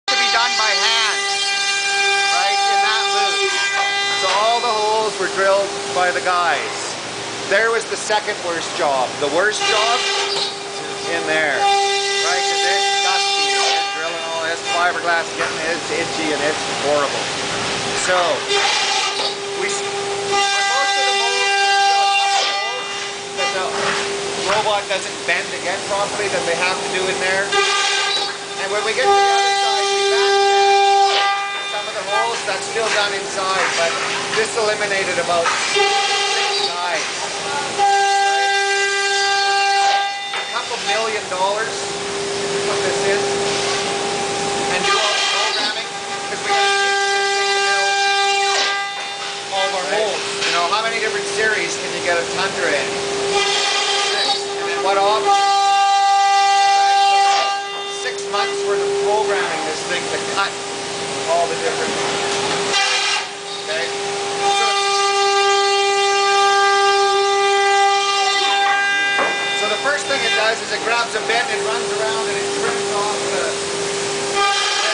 Robot hole cutting spa shell
This is a CNC contolled robot arm cutting holes in a spa shell at the Arctic spas factory